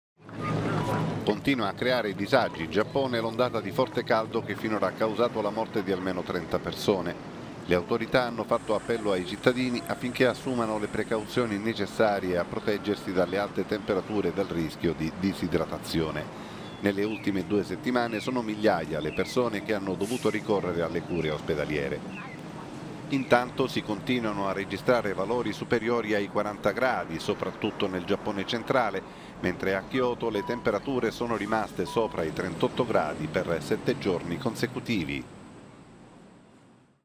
Giappone, continua il forte caldo che finora ha ucciso 30 persone
Migliaia costretti a ricorrere agli ospedali. A Kyoto temperature superiori ai 38 gradi…